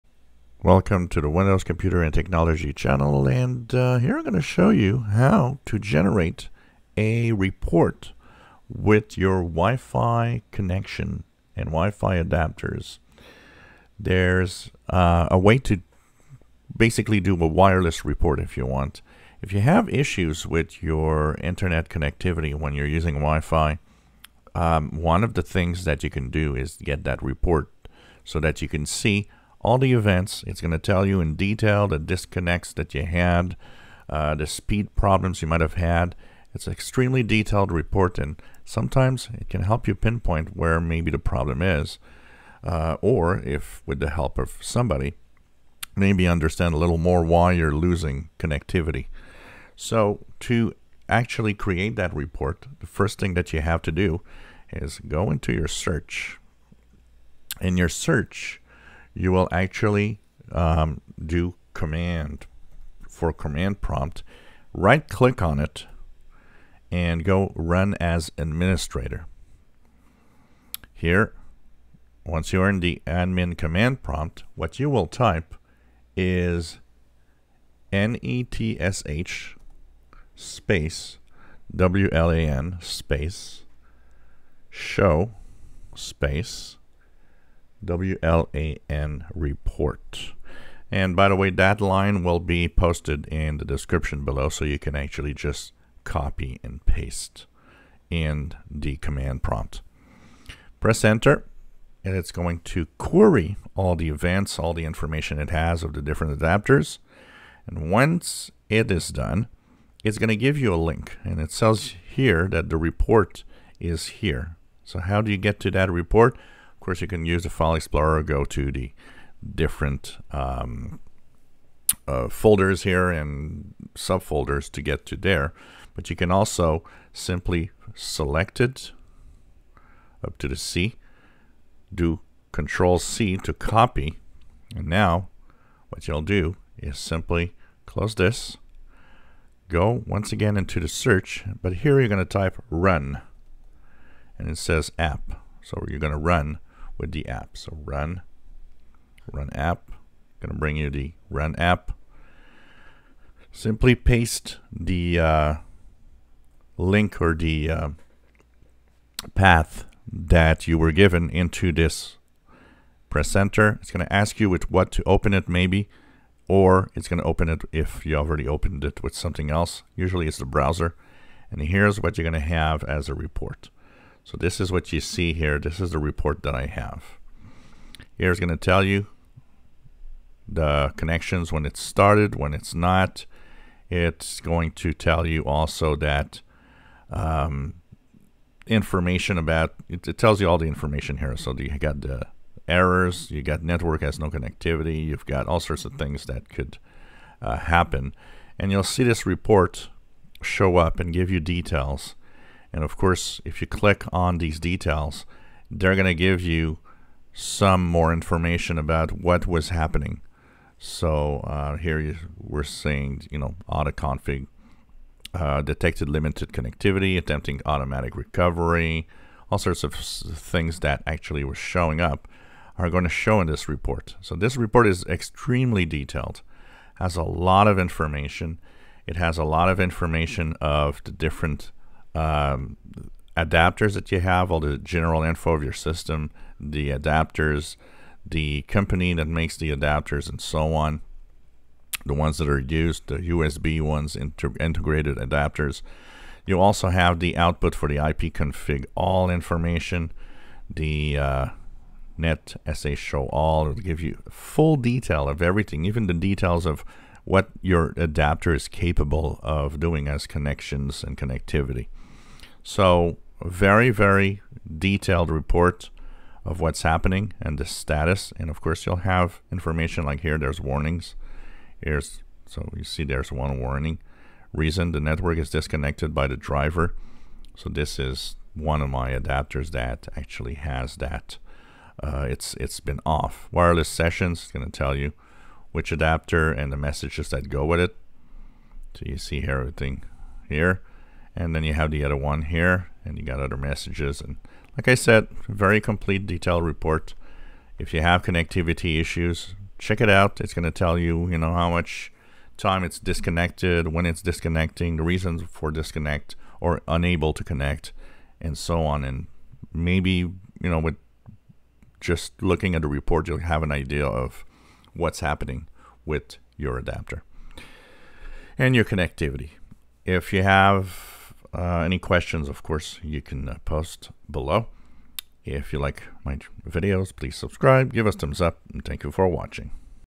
WIFI problems How to create a detailed Wireless report in Windows 11
You can generate a very detailed report of WIFI connectivity if you have some problems

netsh wlan show wlanreport